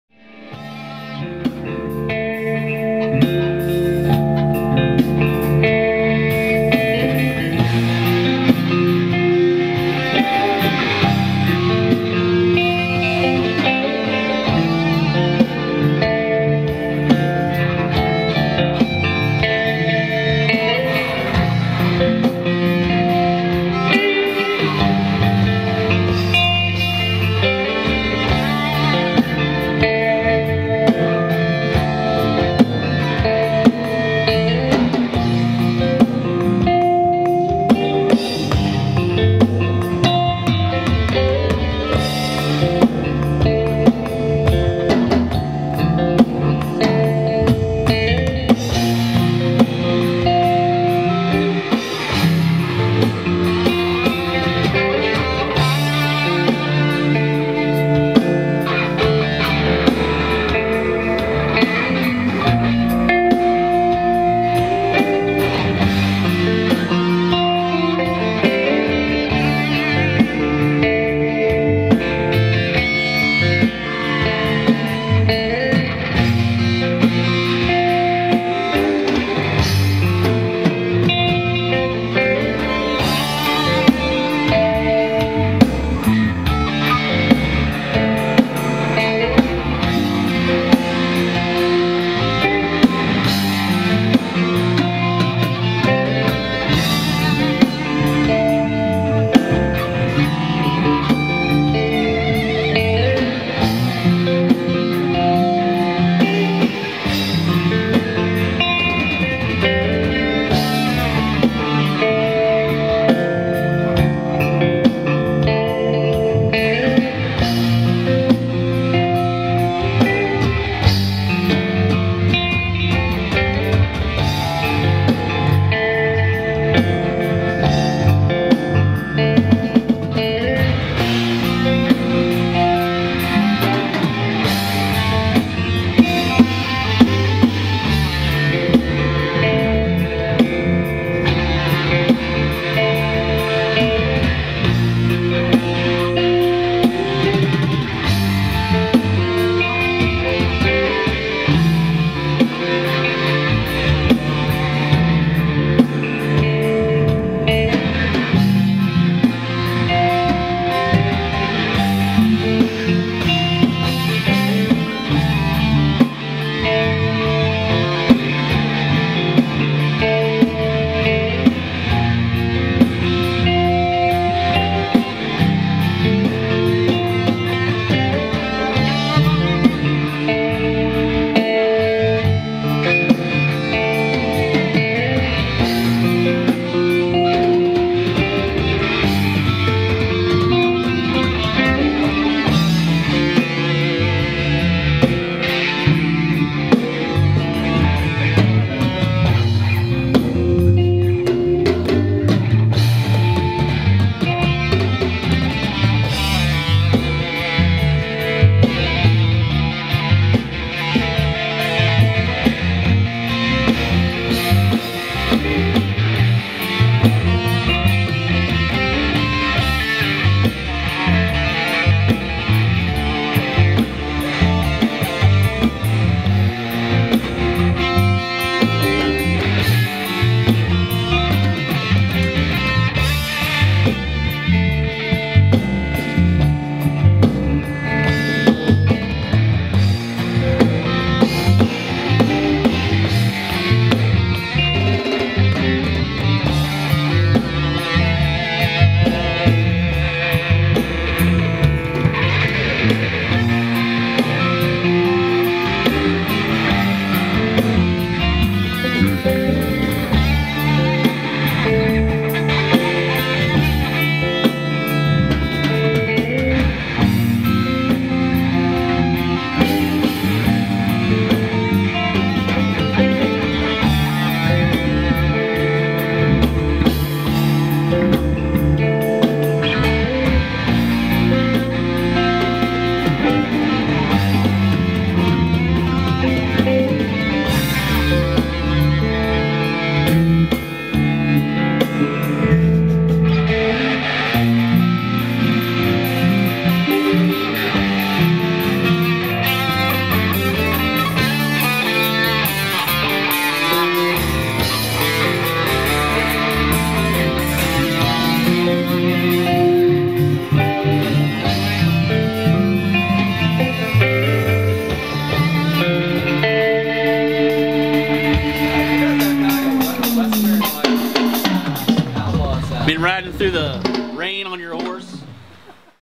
Too Late The Transmission
A few friends of mine have been working out a number of songs for an epic little concept album. I hung out recently during one of their jam sessions.

(Note: I was under the impression this was a song from the album... turns out they were just improvising something based on a snippet from a previous session. not bad for off-the-cuff!)

I have to say they've got some really awesome stuff put together and am really looking forward to the final cut.

For the camera nerds out there:
Shot handheld with a Zacuto Rapid Fire
Canon 7d
Sound, Rode Video Mic Pro plugged through Juiced Link
Graded in Magic Bullet Looks with a custom profile
Slo-mo in Apple Motion